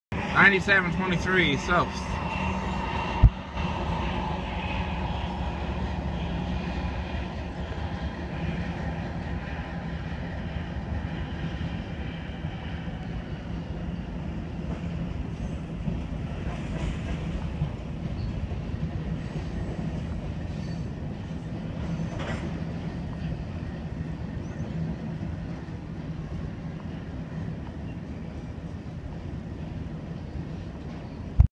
C.P. Southbound Grain.
C.P. southbound grain going into Kipp yard with C.P. 9723. Unfortunately my camera died half way though.